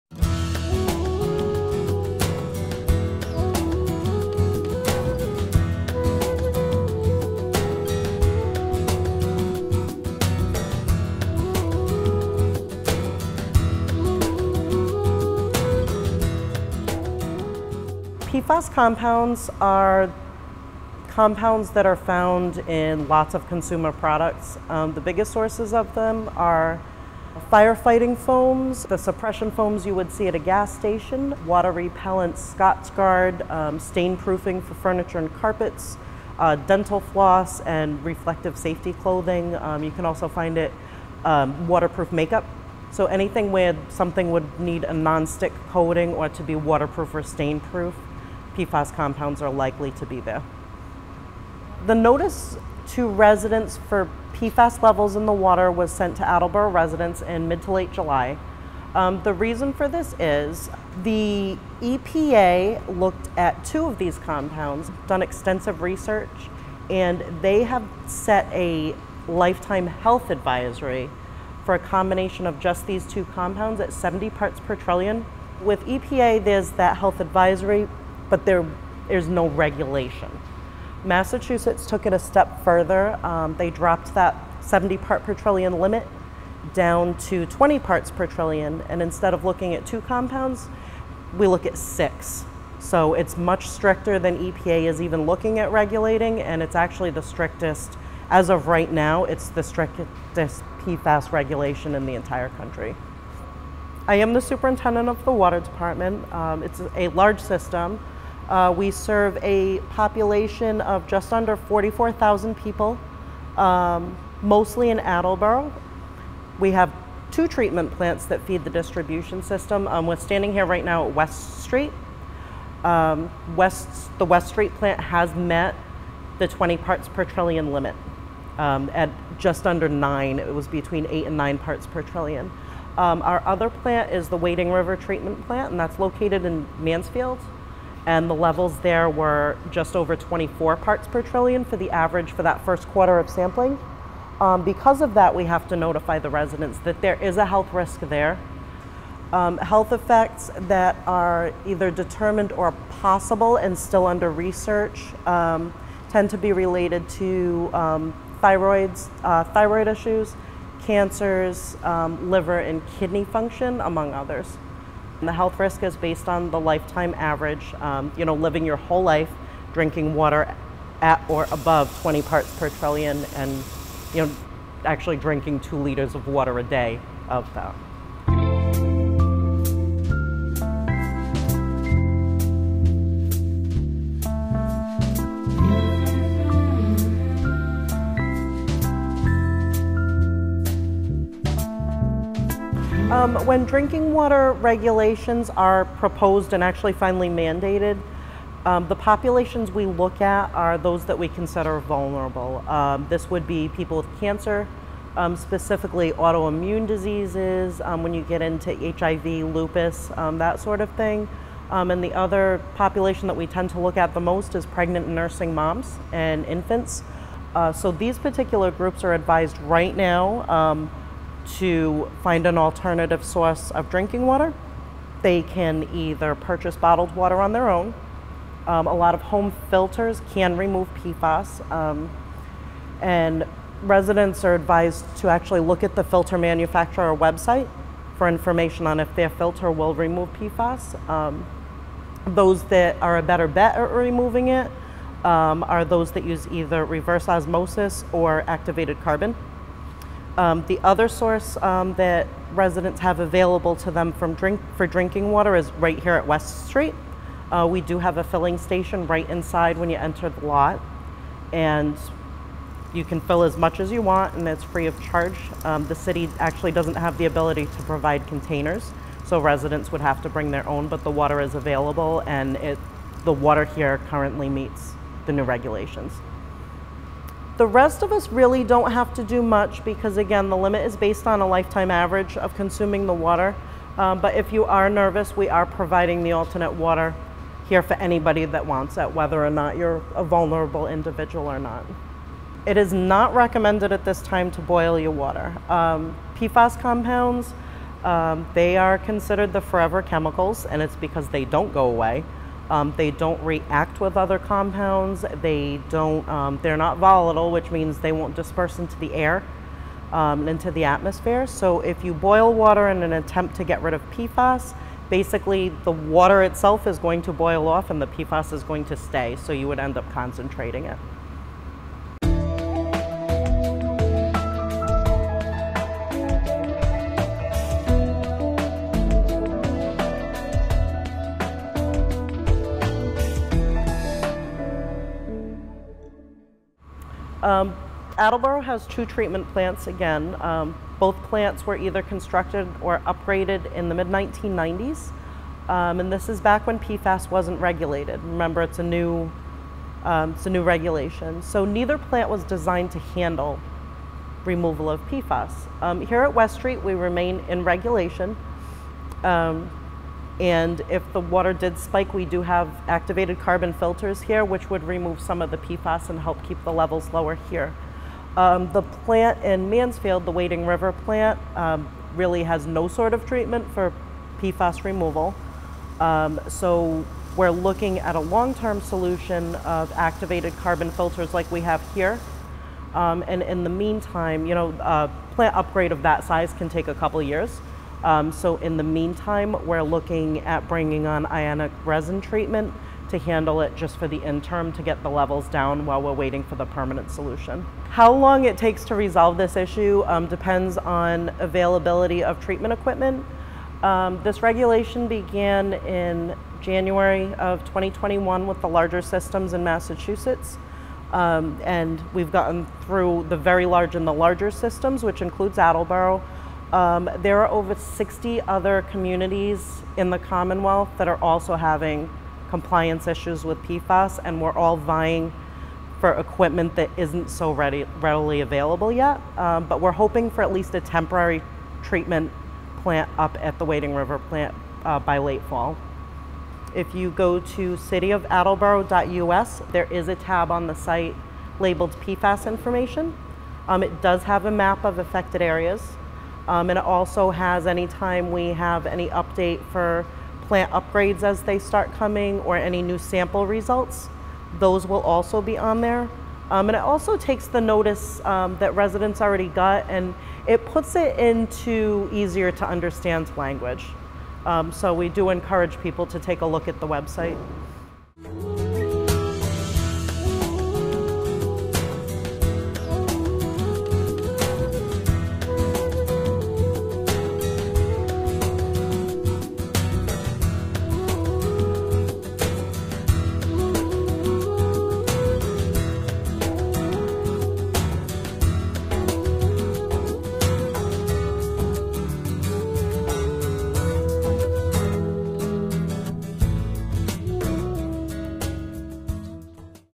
PFAS: The Forever Chemicals
This documentary by @DoubleACS interns Dale Cover and Joey Polis takes a deep dive into the issue of PFAS chemicals in drinking water.